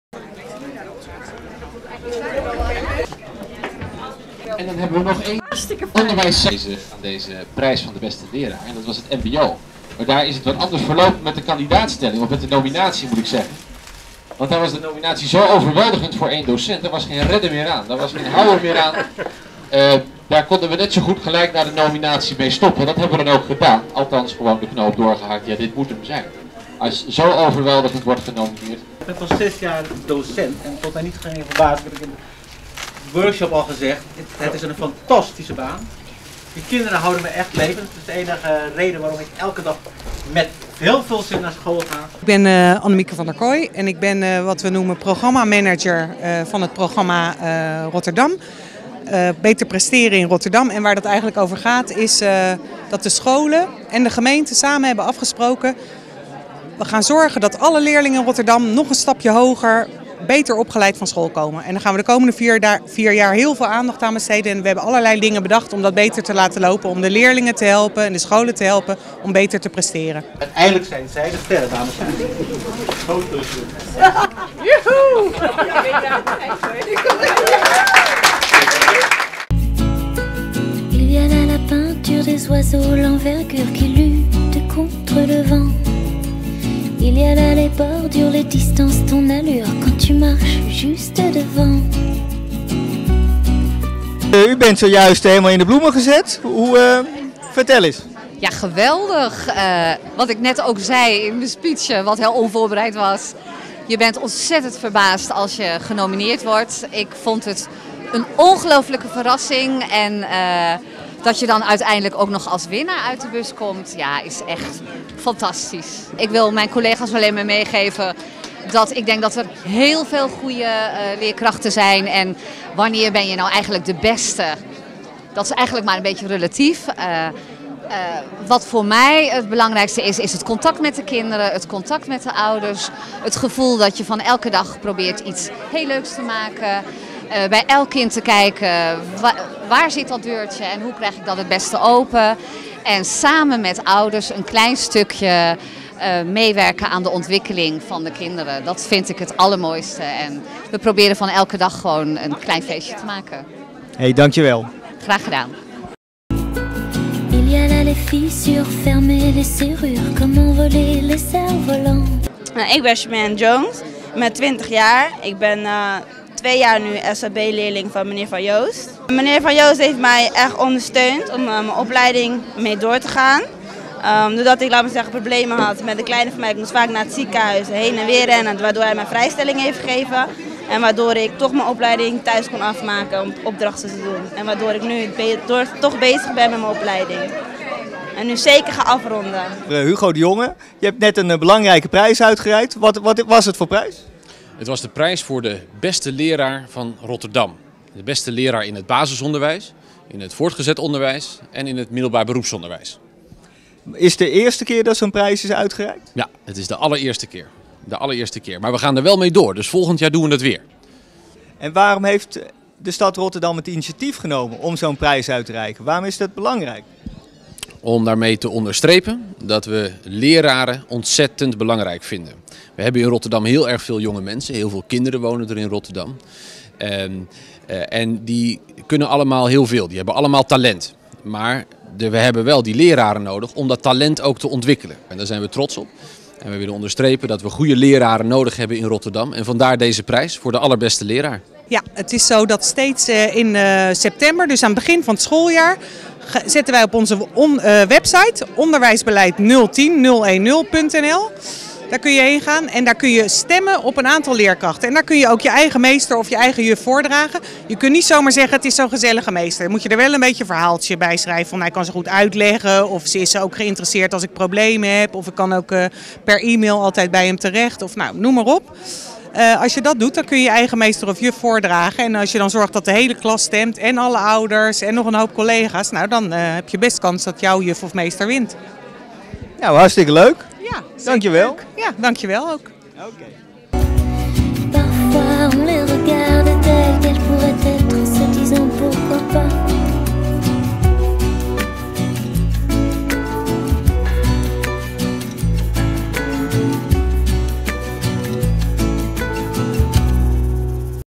Verkiezing besteDocent2001@ Rotterdam
Op woensdag 5 oktober j.l. werd in het educatief centrum Horizon in Schiemond de uitslag van de verkiezing van de beste docent(en) van Rotterdam bekend gemaakt. Dit is een van de initiatieven die de stad Rotterdam vanuit het programma 'Beter Presteren' heeft ondernomen. Er waren drie categorieën; basis (primair) onderwijs, voortgezet onderwijs en middelbaar beroepsonderwijs.
Wethouder de Jonge heeft de officiële prijsuitreiking verricht.